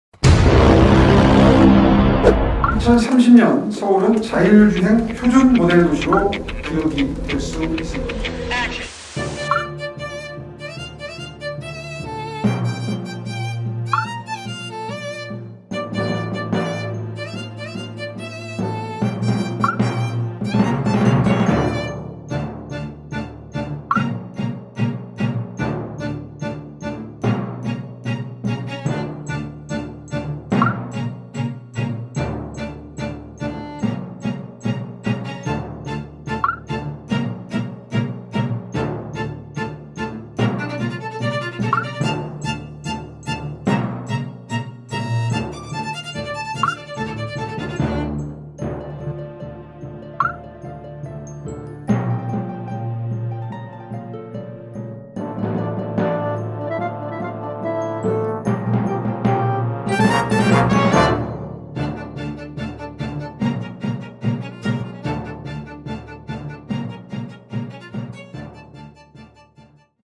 내년부터 청계천에 자율주행 버스 달린다...이거 실화?!
23일 오세훈 서울시장은 `서울 자율주행 비전 2030`을 발표하고 2026년까지 서울 전역 곳곳에 자율주행 인프라를 구축하겠다고 밝혔다.